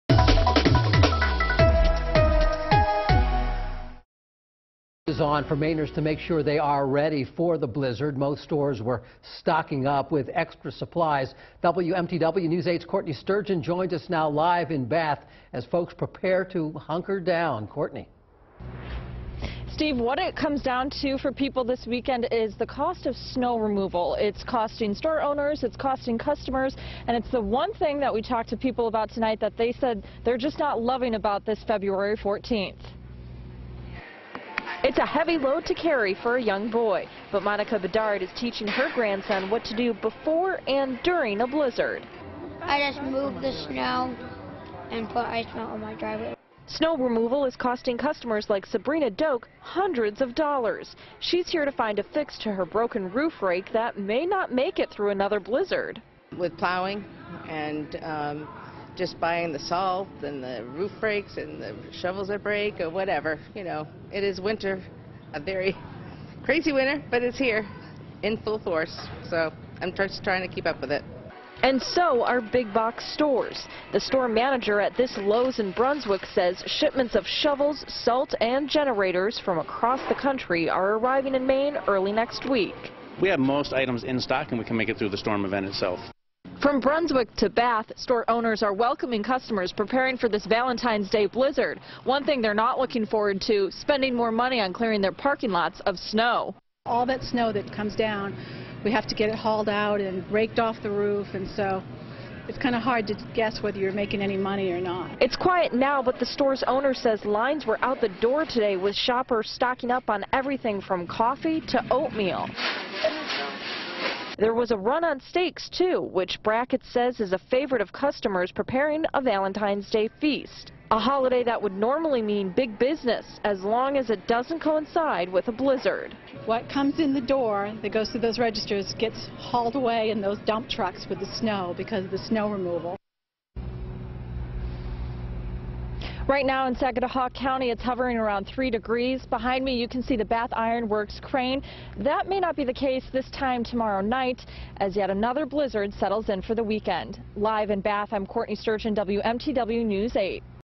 Midcoast businesses talk snow profits, losses
WMTW News 8's Courtney Sturgeon talks to shoppers and businesspeople in Bath and Brunswick about winter expenses. Stores that appear to be raking in sales also have to deal with all the expenses of snow removal.